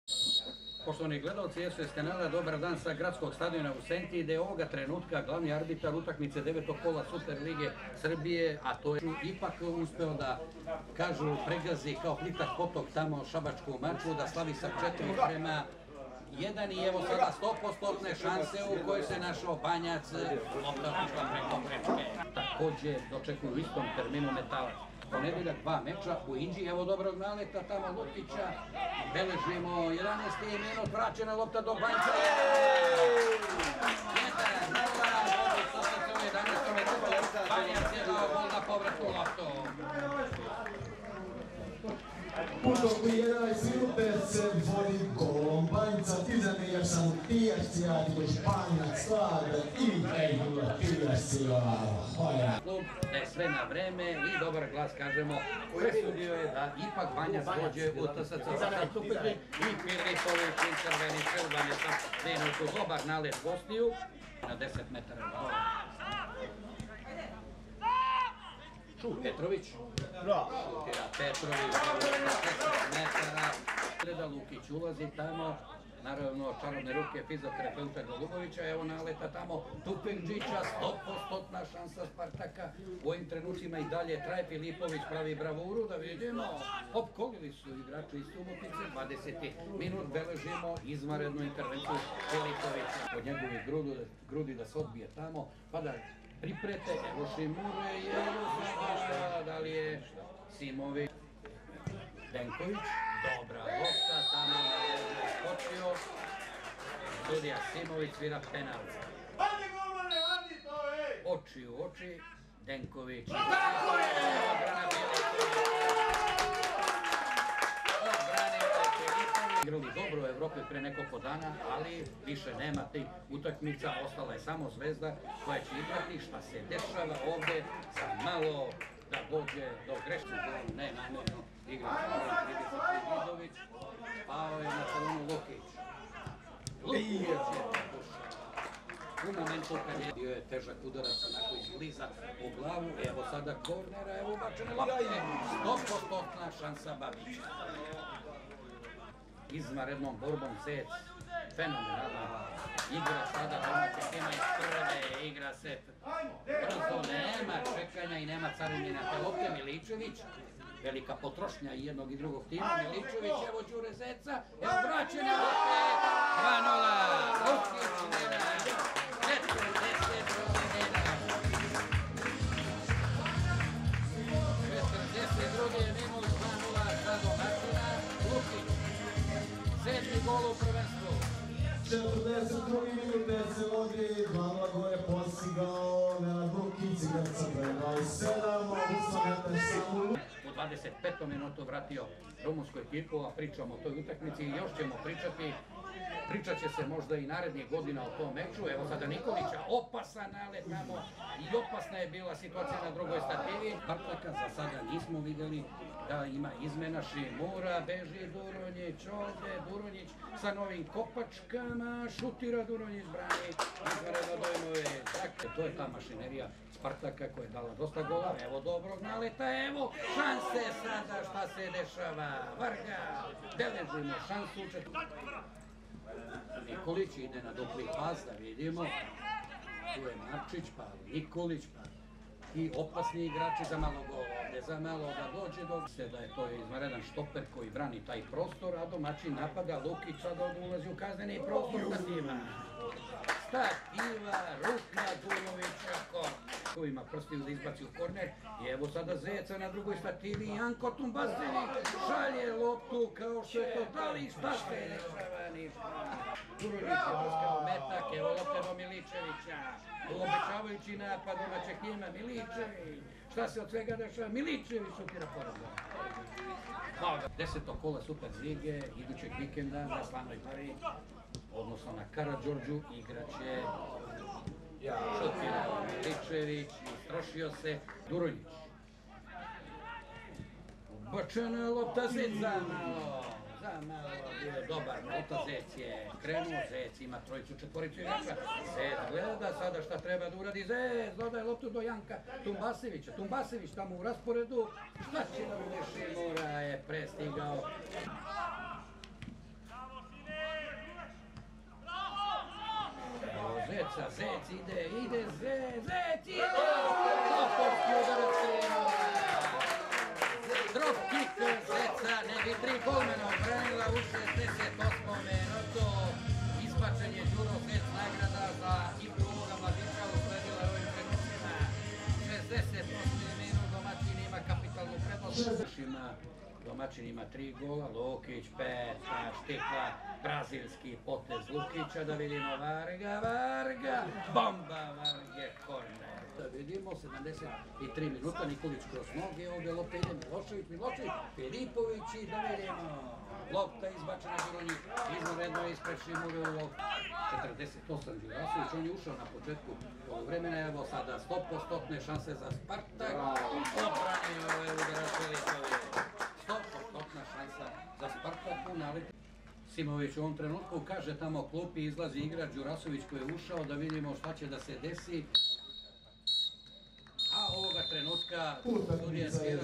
Linglong Tire Super liga 2020/21 - 9.Kolo: TSC – SPARTAK ŽK 3:0 (2:0)
Linglong Tire Super liga Srbije - 9.Kolo: TSC – SPARTAK ŽK 3:0 (2:0)
Strelci: Mihajlo Banjac 11', Nenad Lukič 43', Đuro Zec 68'